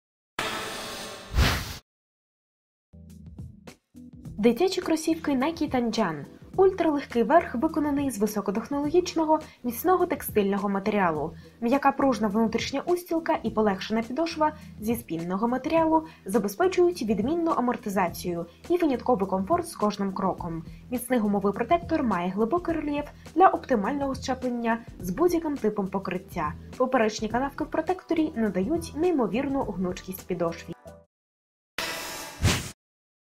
Кроссовки NIKE TANJUN (PS) AS 818385-111
Кросівки Nike TANJUN (PS)

Дитячі кросівки Nike TANJUN (PS) -лаконічний дизайн і функціональність. Назва моделі 単 純 (Tanjun) з японської мови перекладається як простота. Дизайн кросівка замислювався мінімалістичним, без надмірного декору, але при цьому модель демонструє стильний зовнішній вигляд і елегантна у своїй простоті. Ультра легкий верх, виконаний з високотехнологічного, міцного текстильного матеріалу. М'яка пружна внутрішня устілка і полегшена підошва зі спіненого матеріалу забезпечують відмінну амортизацію і винятковий комфорт з кожним кроком. Міцний гумовий протектор має глибокий рельєф для оптимального зчеплення з будь-яким типом покриття. Поперечні канавки в протекторі надають неймовірну гнучкість підошві. Брендинг Nike Swoosh, розміщених на бічних панелях кросівка, доповнює дизайн моделі.

Технології: Flywire; Phylon; Flex Grooves
Матеріл: текстиль / піноматеріал / неопрен / поліуретан
Колір: білий